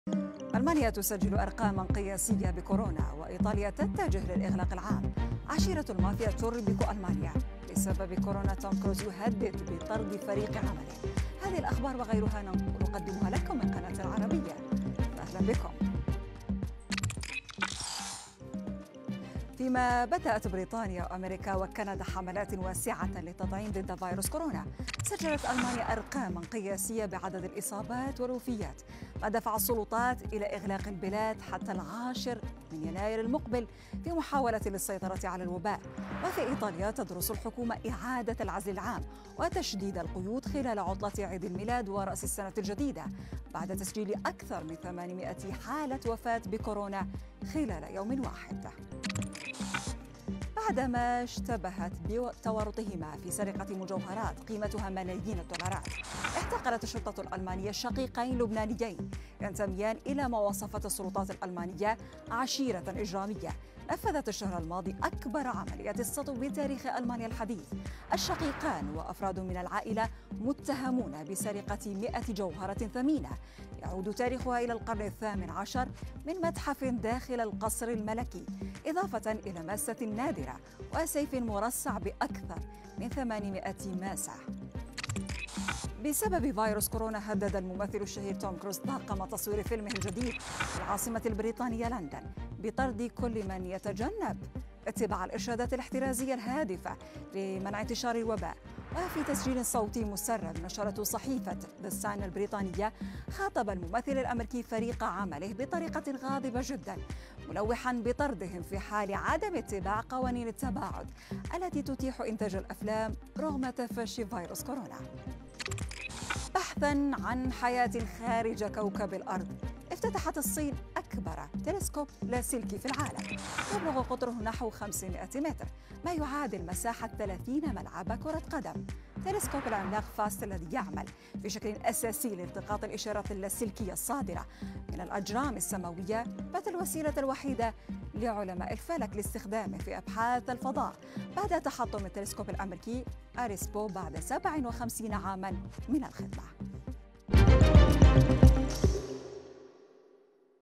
الأخبار في دقيقتين
🇩🇪 ألمانيا تسجل أرقاماً قياسية بكورونا و إيطاليا تتجه للإغلاق العام
💎عشيرة المافيا تربك ألمانيا
🦠بسبب كورونا.. توم كروز يهدد بطرد فريق عمله